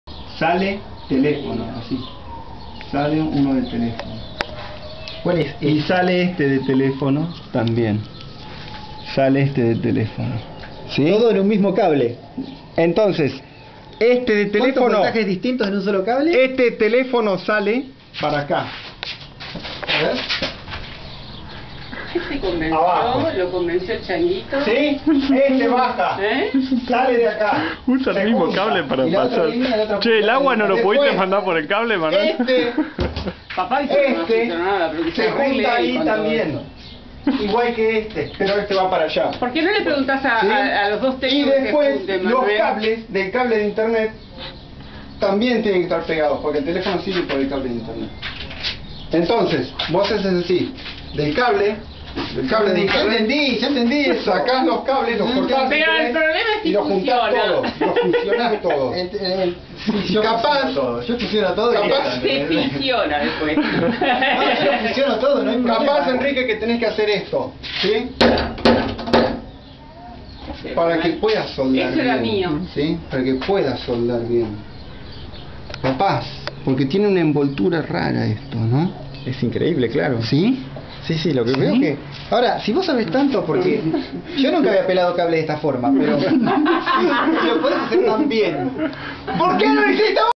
Juan Manuel Cellini y Enrique Daniel Merle discuten de cables mientras TODOS opinan
En el Stud casi nos agarramos a las piñas. Pasamos por el RJ45 telefono, internet y el portero electrico.